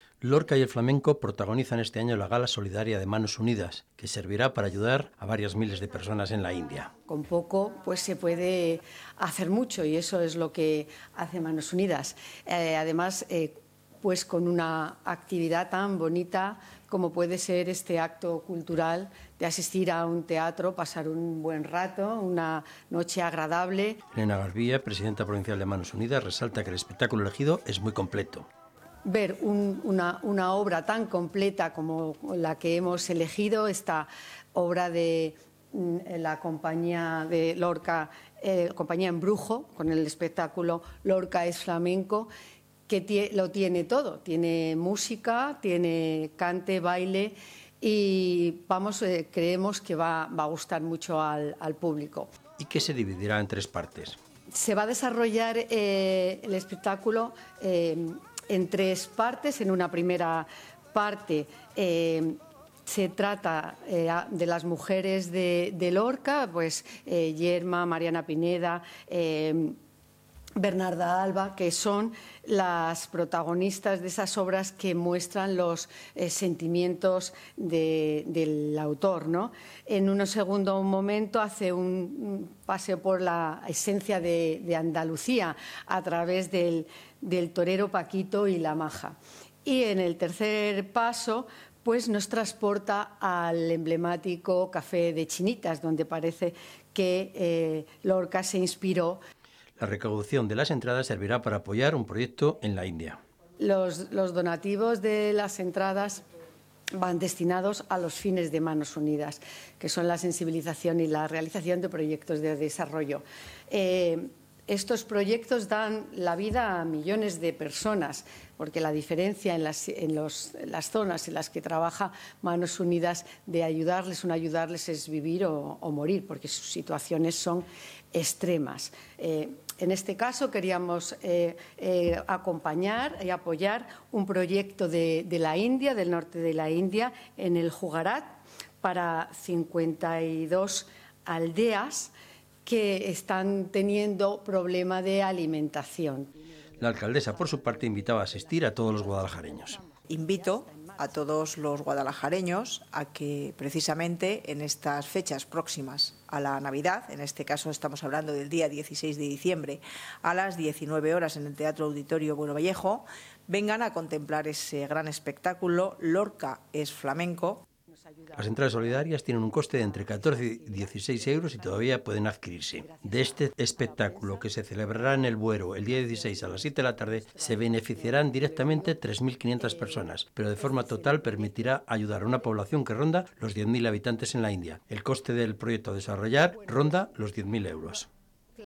Lorca y Manos Unidas
El espectáculo se celebrará el día 16, a partir de las siete de la tarde, en el Buero. Permitirá ayudar a paliar la hambruna en La India